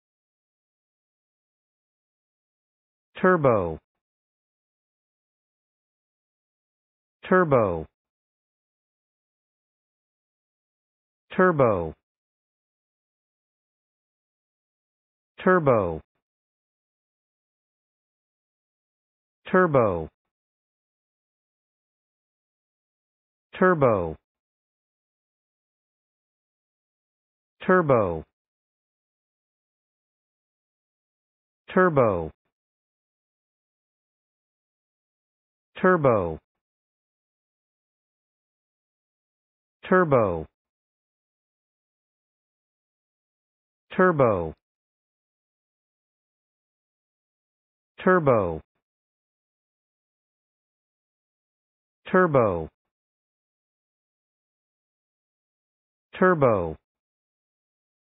Pronunciation of Turbo | Definition of Turbo
Turbo pronunciation | How to pronounce Turbo in English?
/`tɜːrboʊ/
Meaning of Turbo | What is Turbo?
Any one of numerous marine gastropods of the genus Turbo or family Turbinidae, usually having a turbinate shell, pearly on the inside, and a calcareous operculum.
What is Turbo | Turbo meaning and definition?